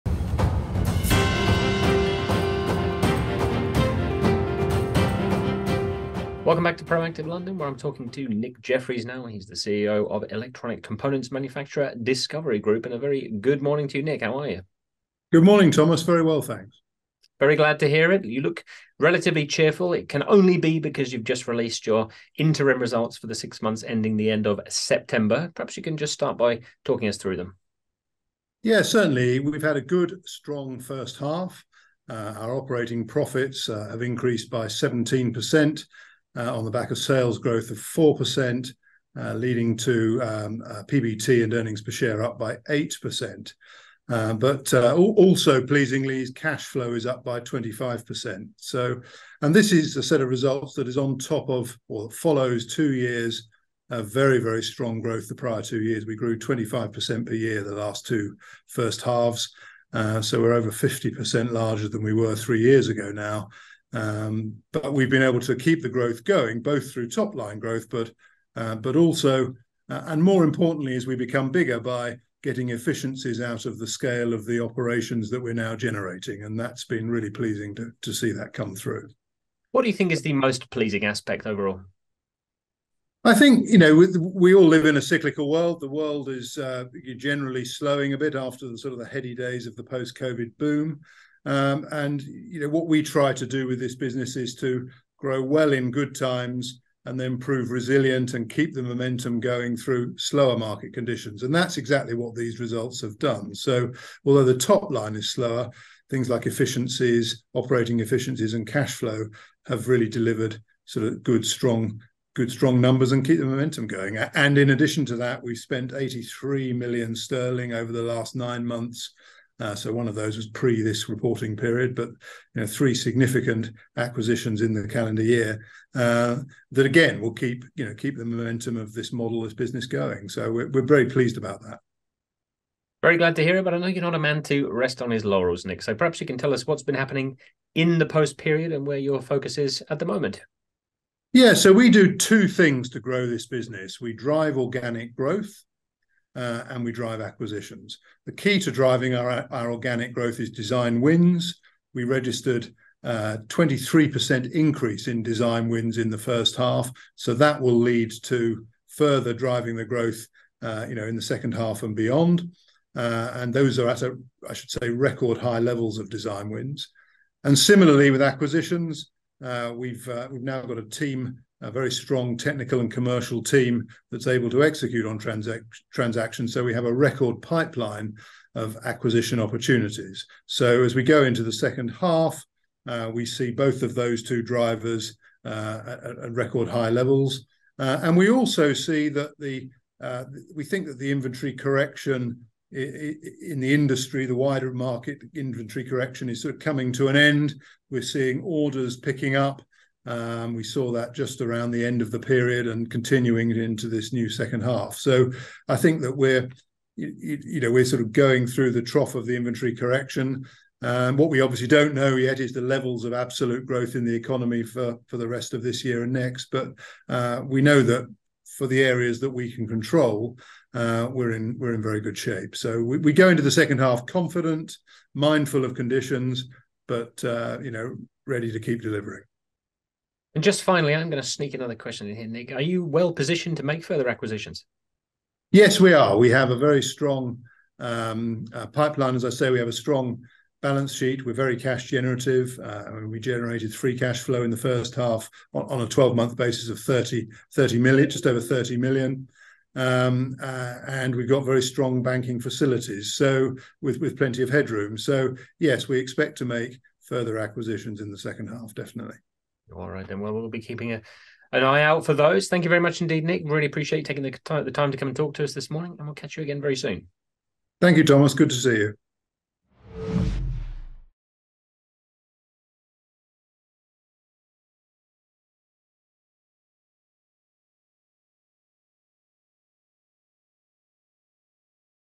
DiscoverIE Group CEO hails profit increase
DiscoverIE Group PLC (LSE:DSCV) CEO Nick Jefferies speaks to Thomas Warner from Proactive London after the electronic component manufacturer released interim results for the six months to 30 September 2023 showing an increase in profitability.

Jefferies gives an overview of the figures, highlighting a 17% increase in operating profits on 4% sales growth. This success translates into an 8% rise in both profit before tax and earnings per share, alongside a significant 25% boost in cash flow.

Jefferies also highlights the company's sustained growth trajectory, having expanded by over 50% in the past three years. He attributes this success to a balanced strategy of driving top-line growth and achieving operational efficiencies as the company scales up.

Despite what he calls the cyclical nature of the market and a general economic slowdown post-COVID, discoverIE has demonstrated resilience and continued momentum, particularly through operating efficiencies and robust cash flow.

Looking ahead, Jefferies expressed confidence in the company's position, with a focus on navigating through the industry's inventory correction phase and continuing to drive growth. He also affirms the company's readiness for further acquisitions, supported by a strong balance sheet and ample banking facilities, expecting more acquisitions in the latter half of the year.